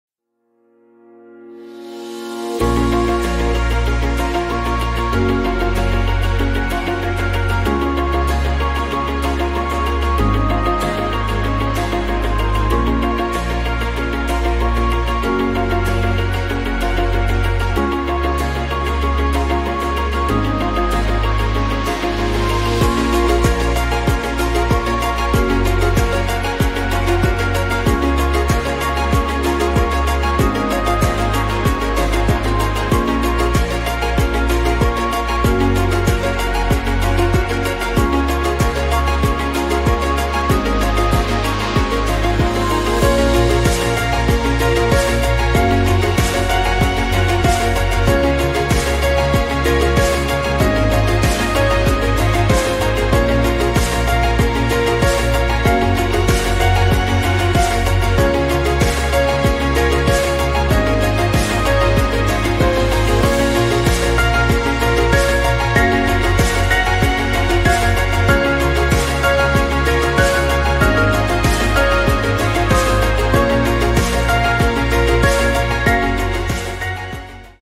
Why is it critcal to price your home right? Seller's or Buyer's market, doesn't matter...
🤩🤩 [Seller's or Buyer's market]: 💥💥 You gotto price a home right. 💲💲 Here are a few insights... 🌈🌈